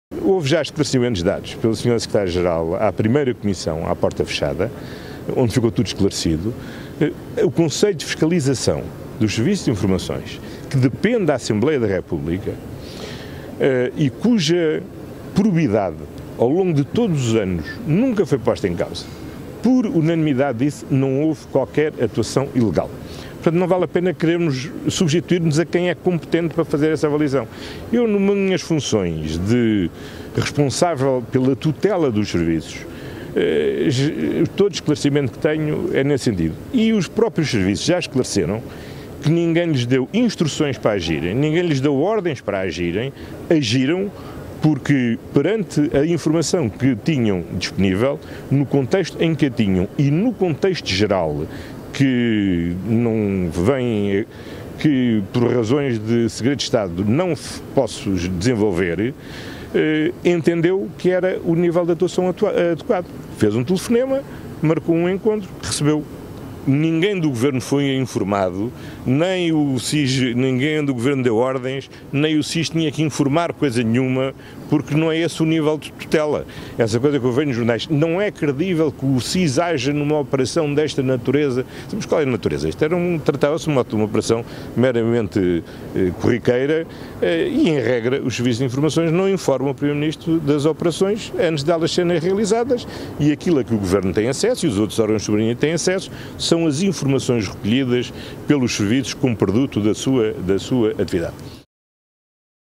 Costa rejeita contradição de versões com Galamba e defende secretária-geral do SIRP
O primeiro-ministro rejeitou hoje haver contradição entre a sua versão de que não teve conhecimento prévio da atuação do SIS na recuperação de um computador do Ministério das Infraestruturas e as declarações do ministro João Galamba.